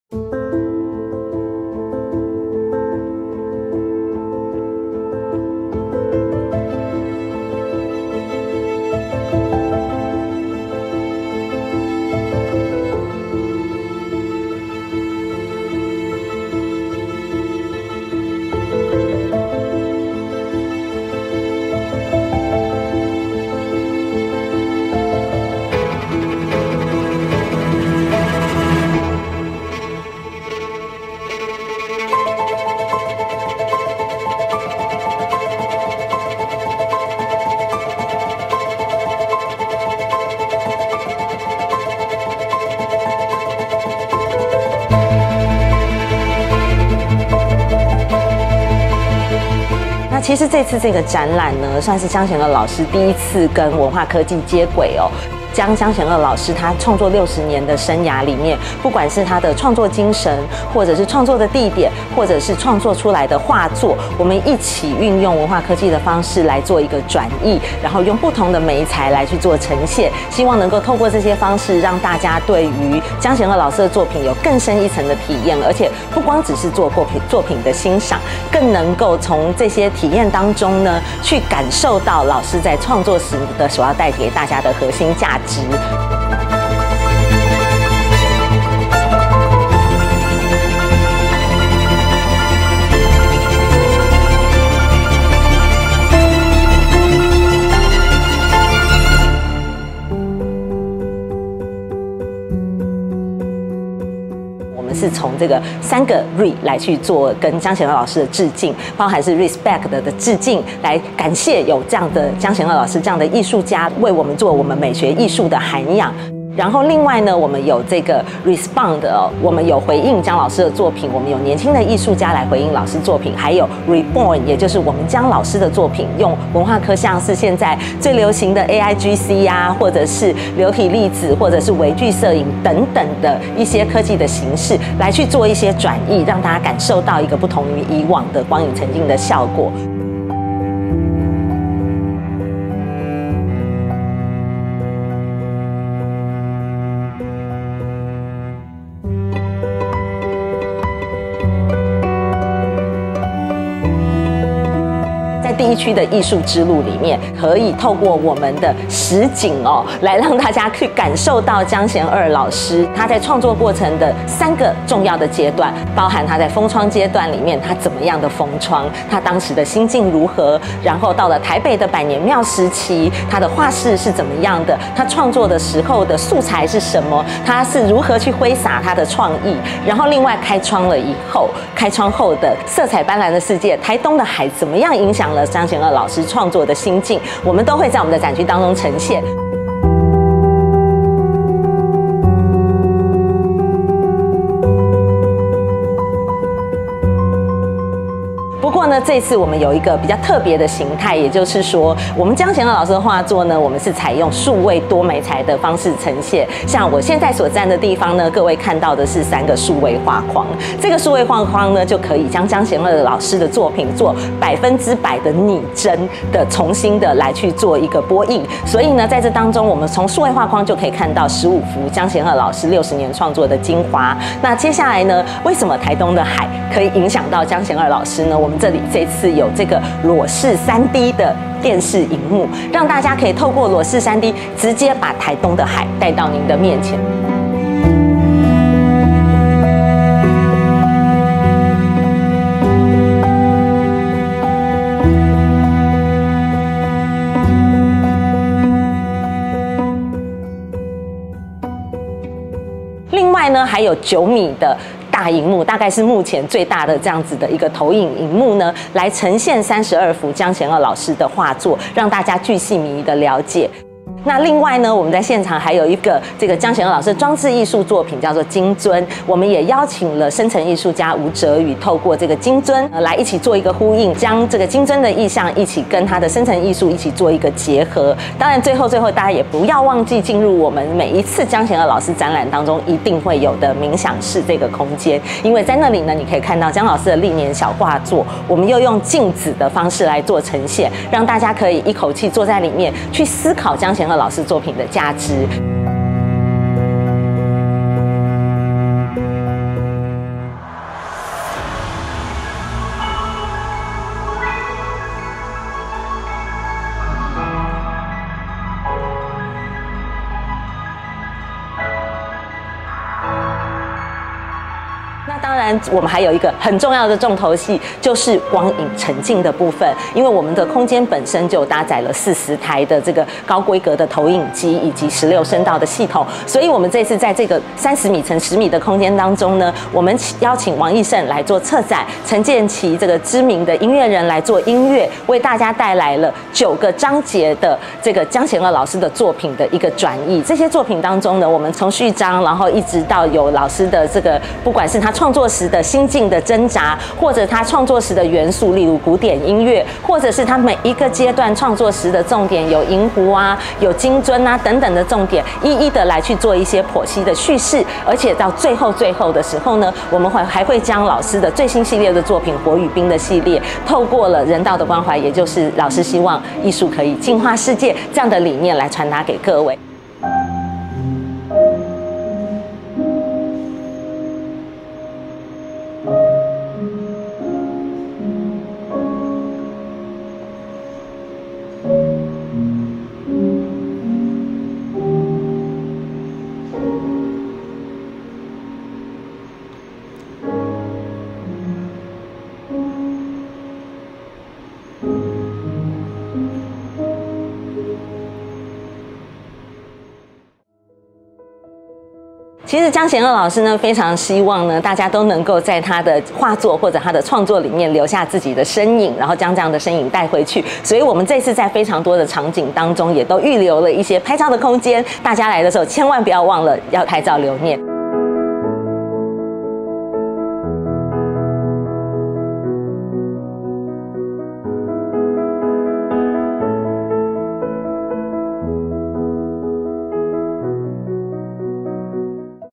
走進當代藝術大師的畫作！《RE：江賢二》數位冥想．江賢二光影沈浸展 | 鮮貨報報 | 壹起FUN
《RE：江賢二》數位冥想．江賢二光影沈浸展
以劇場敘事手法梳理江賢二60年藝術創作旅程
透過融合前衛 AI 與生成藝術跨域手法
將江賢二畫作轉譯成全沈浸數位體驗
將藝術帶入全新的數位境界！

展題中的「Re」不僅是對江賢二的「致敬」（Respect）
更將「Re：江賢二」定義為創作
引「光」為敬，提煉出九個環繞的主題
主視覺由何庭安設計
並邀請7組國內頂尖新媒體設計團隊
（王奕盛、谷汩文化、有日互動、動工設計、叁式、珀石超媒、吳哲宇）
及重量級音樂製作人陳建騏合力創作
共同解構73幅江賢二的作品以及其創作精神
打造九段富含詩意且風格迥異的數位冥想
並融合AI演算與生成科技的語彙
重現隱藏於江賢二作品後的創作內涵
非常值得一看的光影沈浸展！

■ 展覽期間：2023/12/22(五) ~ 2024/03/03(日)
■ 展覽地點：双融域 AMBI SPACE ONE
■ 展覽地址：台北市信義區信義路五段7號5F
            （台北101觀景台入口處旁）
■ 展覽時間：每日11:00–18:00

下載【年代電視】APP：
iOS平台：